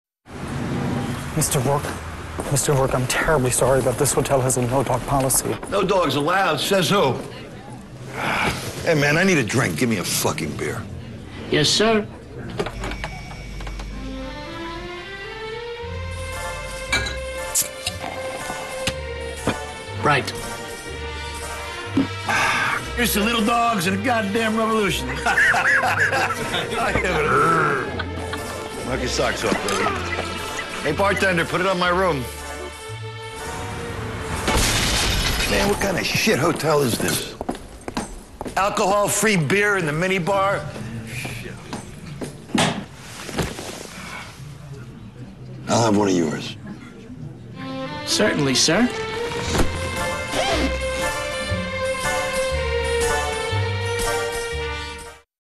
Bavaria hires Rourke to sell alcohol-free beer
This commercial for Bavaria 0% was created by Selmore and directed by Jonathan Herman.